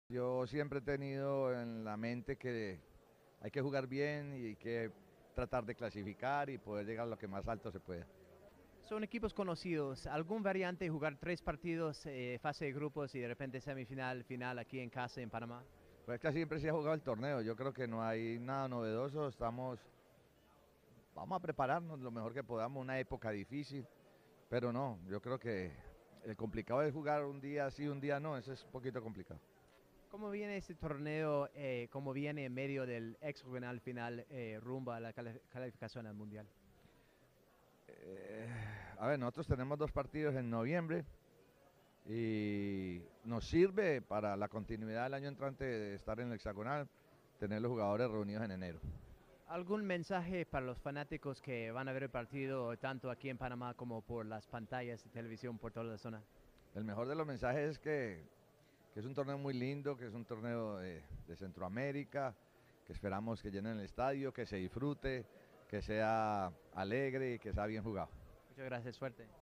Hernán Darío Gómez, DT Panamá
Vamos a prepararnos de la mejor manera para ganar el título Hernán Darío Gómez, DT @Fepafut